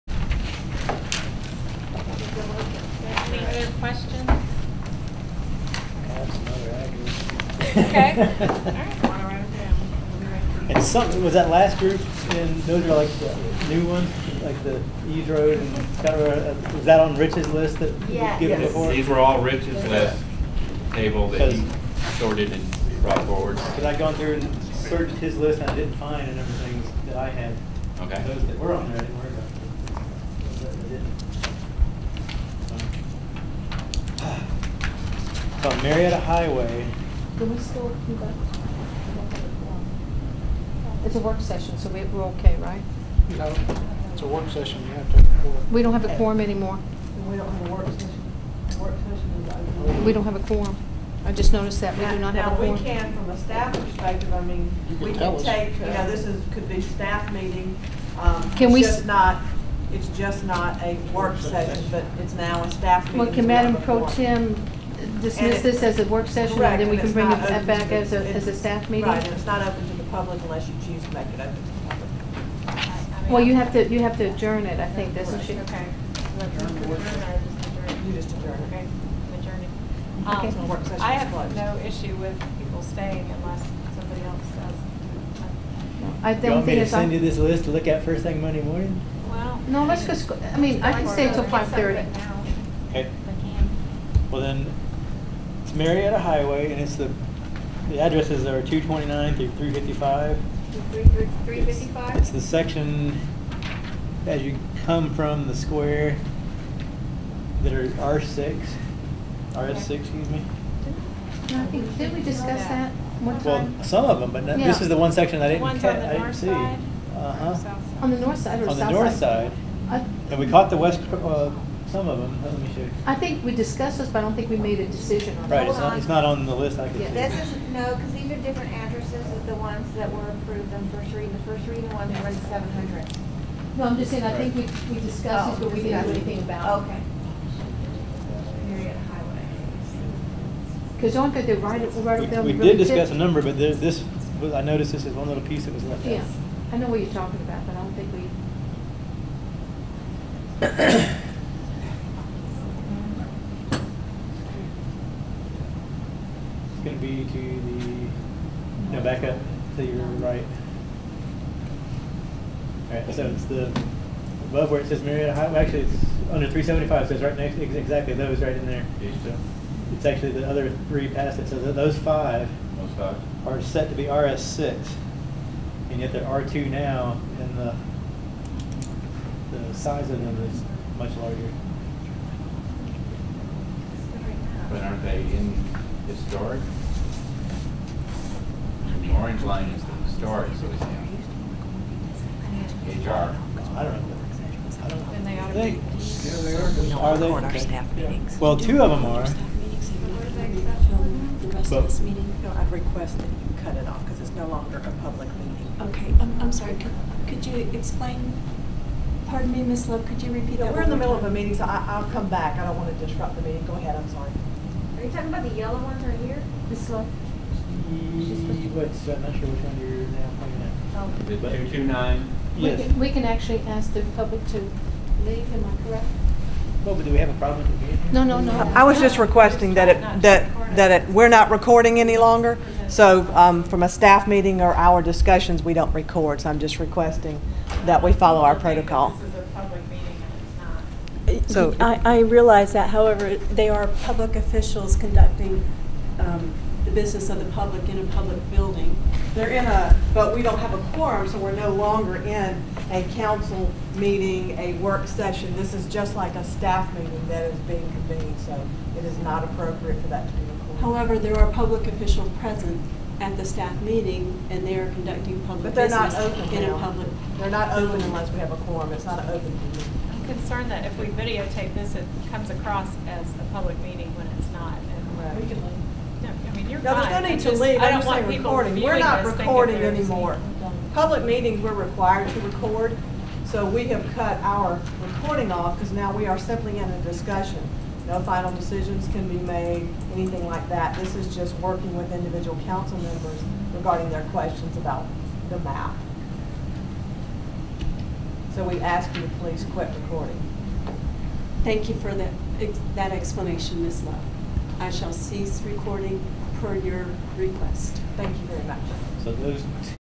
City of Roswell requests Nydia Tisdale’s video camera be turned off 02/08/14
Roswell City Manager Kay Love and Mayor Pro Tem Nancy Diamond request that citizen journalist Nydia Tisdale turn off her video camera when a quorum is lost during an open-and-public-City-Council-work-session which is renamed a closed-staff-meeting-with-the-public-permitted-to-attend.

City of Roswell
38 Hill Street

Nydeo by Nydia Tisdale for AboutForsyth.com.
Select 1080p for HD quality viewing.